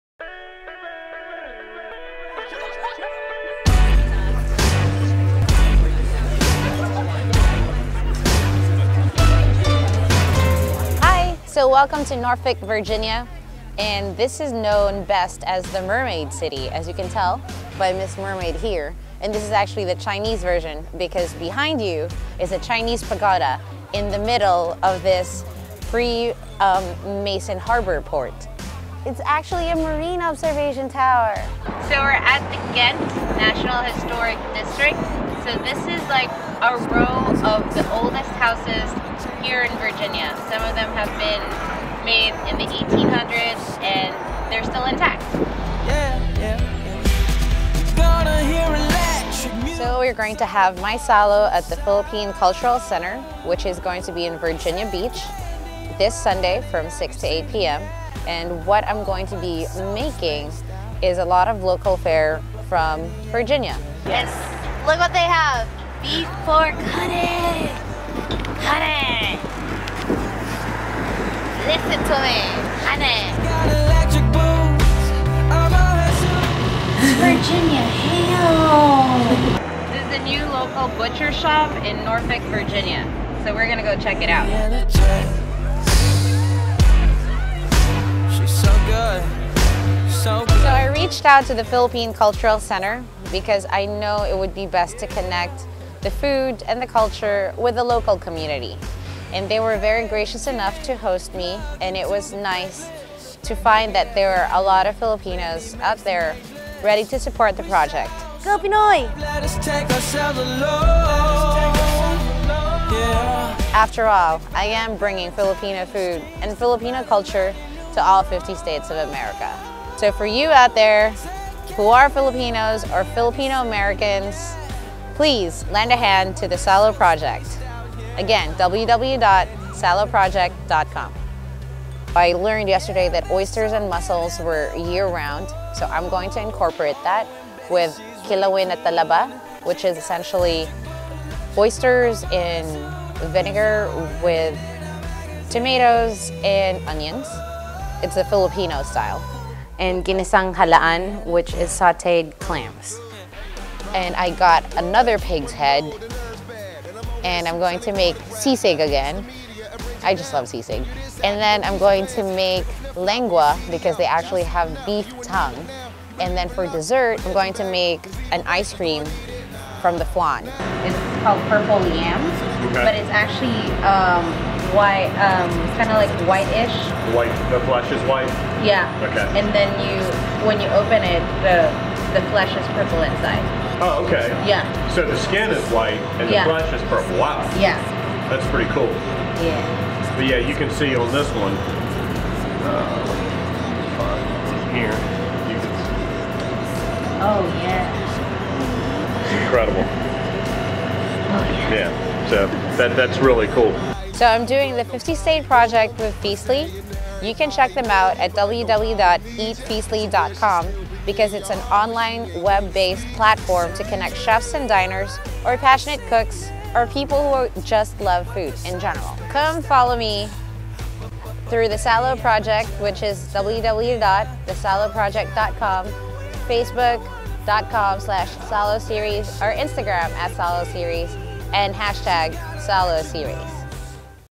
SALO Virginia
SALO in the First State of the United States: Virginia with Feastly Hosted at the Philippine Cultural Center.  Meat from Pendulum in Norfolk, Oysters from Pleasure House Oysters.

Donate to the SALO Project bringing Filipino food to all 50 states in 50 weeks